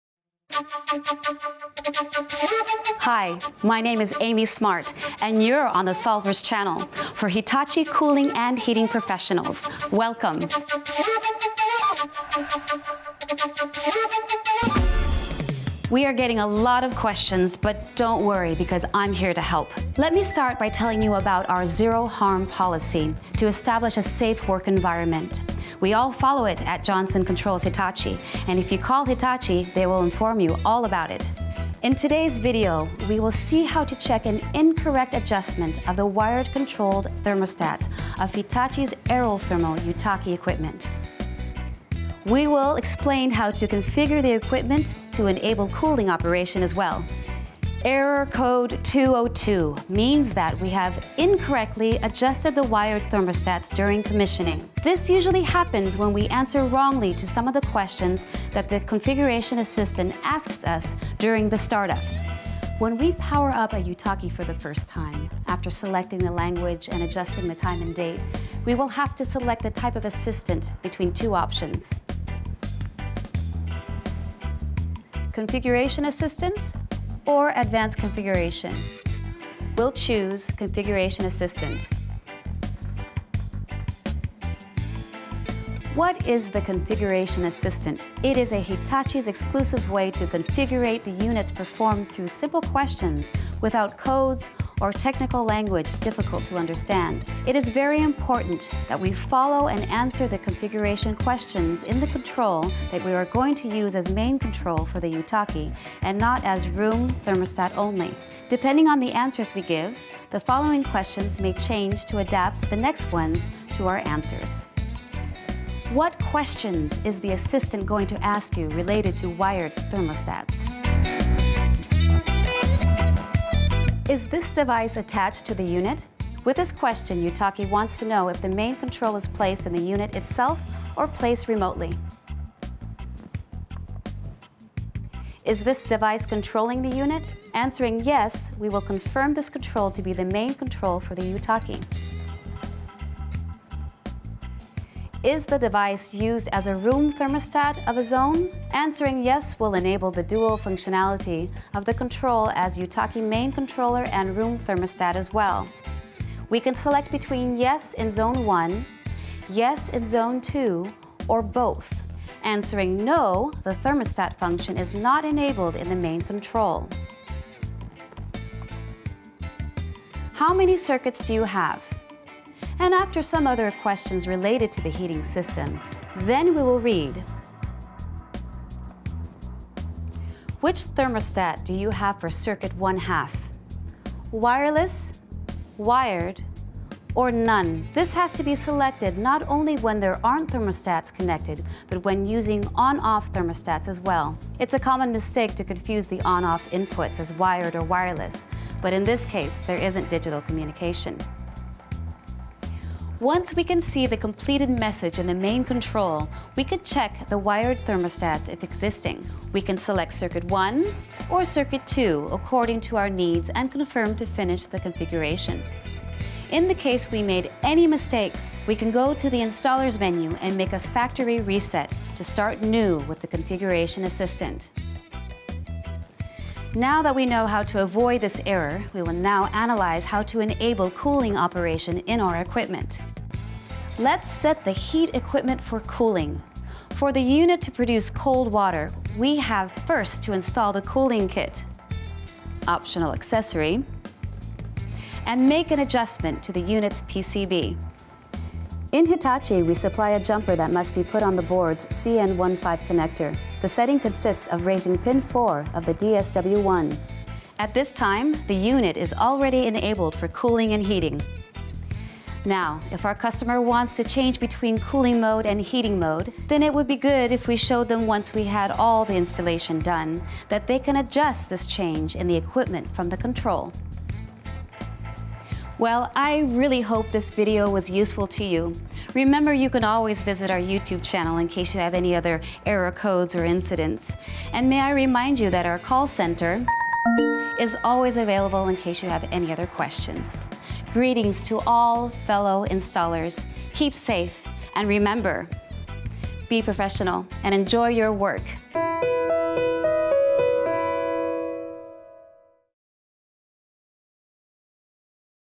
How to solve Hitachi 202 error code on Yutaki equipment
Discover how to check an incorrect adjustment of the wired controlled thermostat of Hitachi's aerothermal Yutaki equipment with The Solvers!

Error code 202 means that we have incorrectly adjusted the wired thermostats during commissioning.

This usually happens when we answer wrongly to some of the questions that the Configuration Assistant asks us during the startup.

Besides we will explain how to configure the equipment to enable cooling operation as well.

Have The Solvers helped you fix and resolve the error?